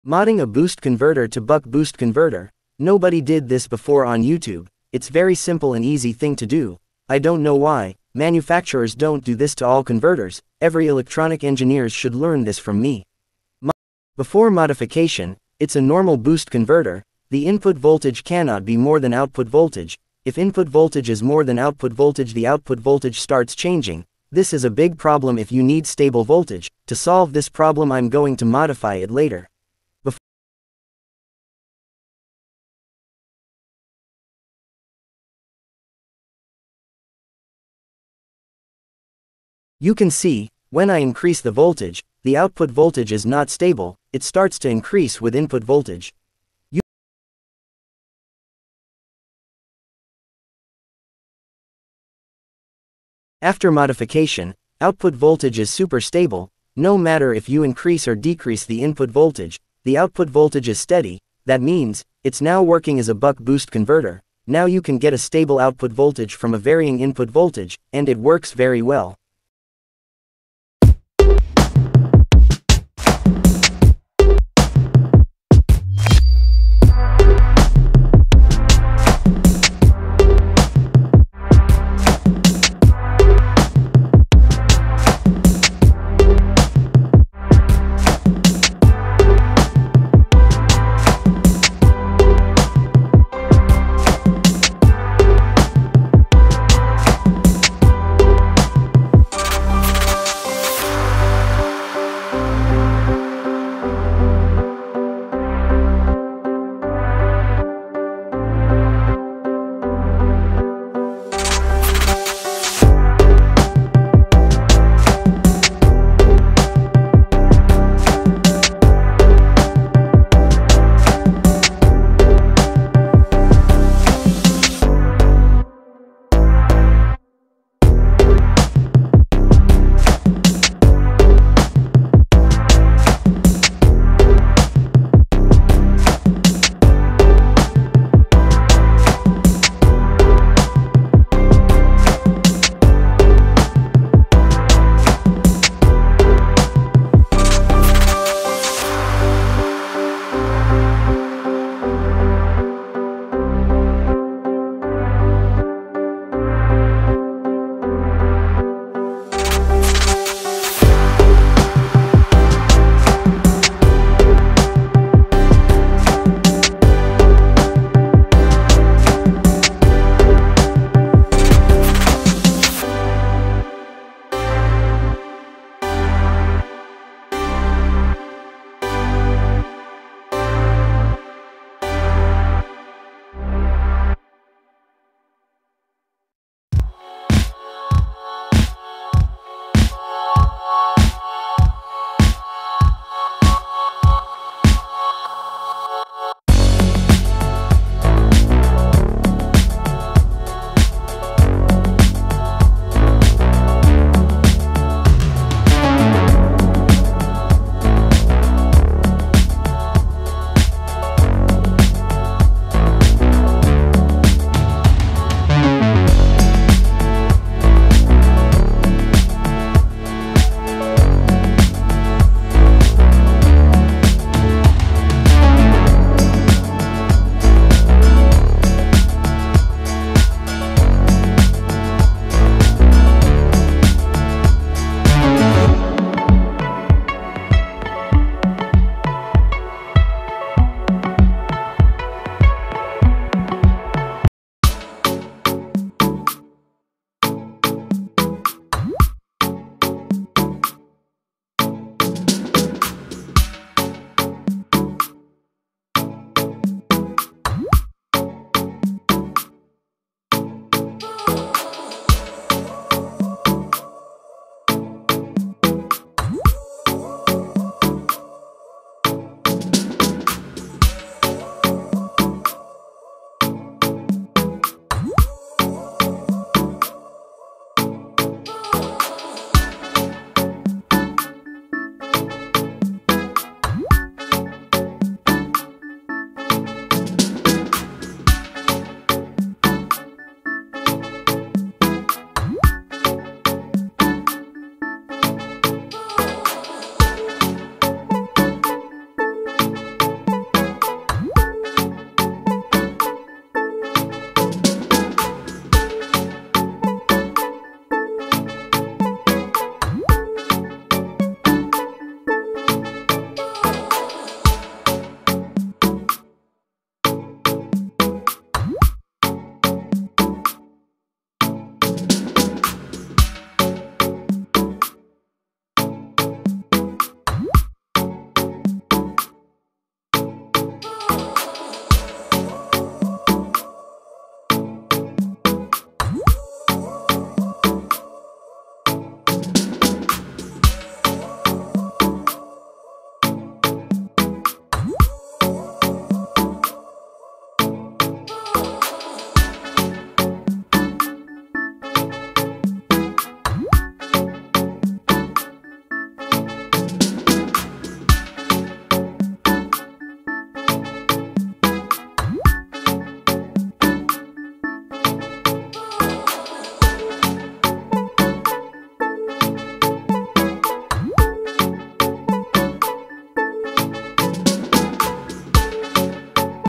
How to Modify your normal boost converter to Buck Boost
Join me on a journey through the world of electronics as we tinker with a boost converter, transforming it into a versatile buck-boost mode. This isn't your typical tech talk; we'll unravel the process step by step, exploring the why and how behind the modification. Whether you're a seasoned enthusiast or a curious beginner, there's something exciting for everyone in this DIY adventure. Grab a seat, and let's dive into the fascinating realm of circuit modifications together!

So, if you're ready for a boost of DIY inspiration and a dash of electronics magic, hit that play button, join the adventure, and let's turn this boost converter into a buck-boost marvel! Until next time, happy tinkering!

0:01 Start
0:07 Before and after
0:14 Before modification
0:51 After Modification
1:50 Start Modification
1:55 Diode
3:00 Inductor
3:25 Re-wind Inductor
4:30 Modified
6:00 Modification Complete
6:10 End